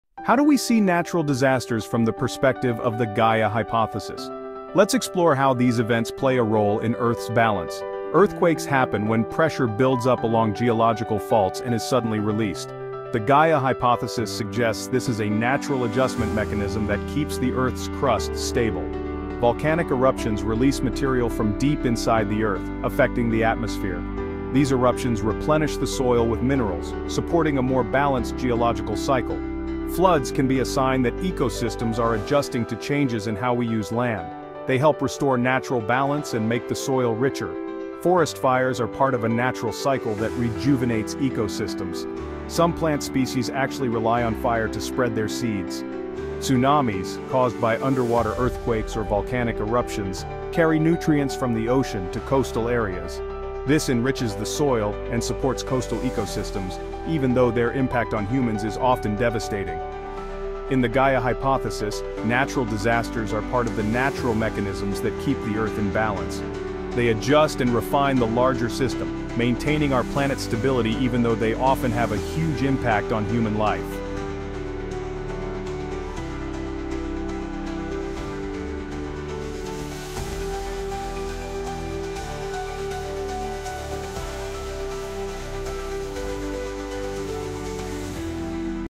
How the Gaia Hypothesis Explains Natural Disasters
In this video, we explore the Gaia Hypothesis and how this theory explains the connection between Earth and natural disasters. The Gaia Hypothesis suggests that Earth functions as a self-regulating system, where all elements, from the atmosphere to living organisms, work together to maintain a balance that supports life. We’ll also dive into how disruptions in this system can trigger natural disasters like earthquakes, tsunamis, and storms. Discover how the Gaia concept can help us understand natural disaster phenomena in a more holistic way, and how we, as humans, can learn from nature to achieve better balance